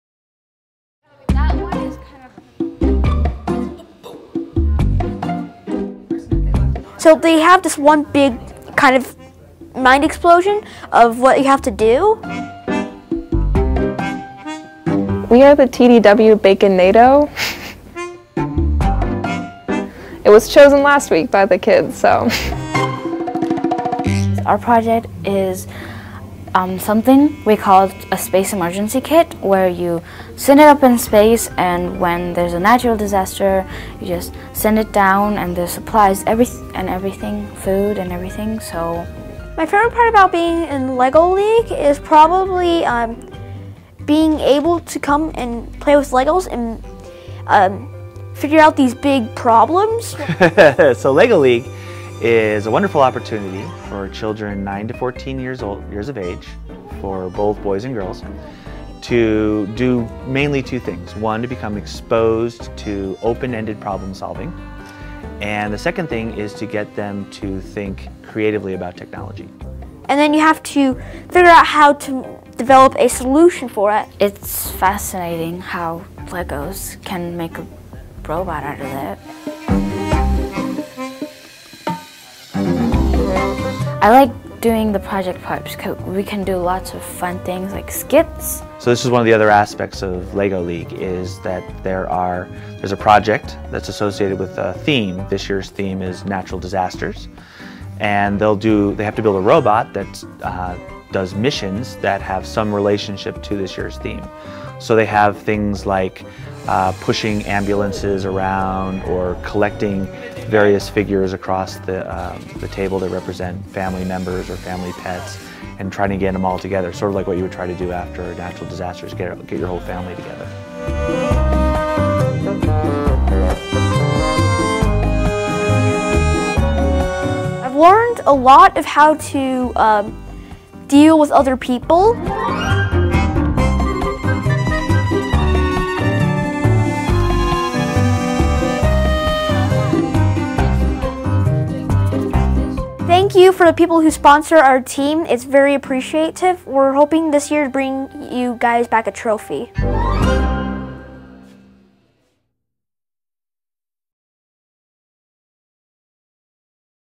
LEGO®s, Robots, and the Future of the Industry
Paring professionals with students to engage the next generation of engineers, FIRST® LEGO® League robots battle the fallout of a natural disaster and teach children how engineering can empower them to shape the world's future.